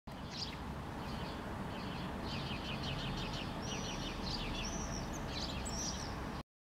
Blue Tit perching, taking off
Blue Tit (Parus caeruleus) perching on a wire, then taking off.

Sinitiainen (Parus caeruleus) istuu katulampun vaijerilla; lähtee lentoon.